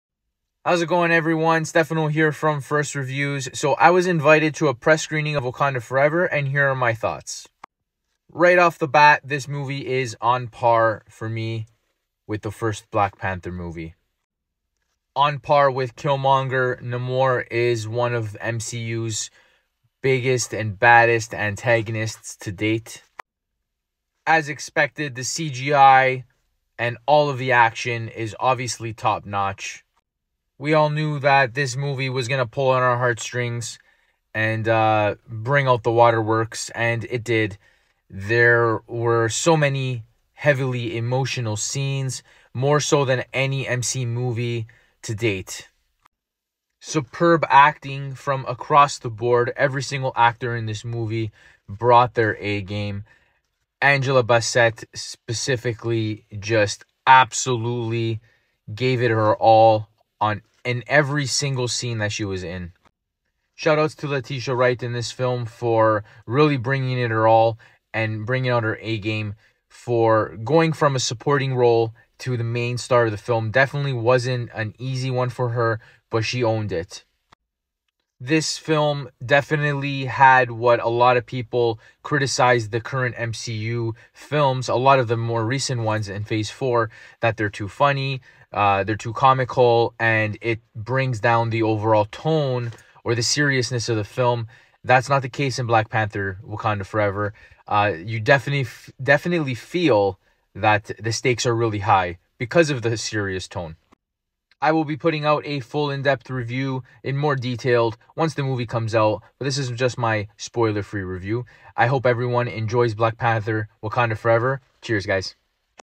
Wakanda Forever
Spoiler free review of Wakanda Forever after checking out a press screening this week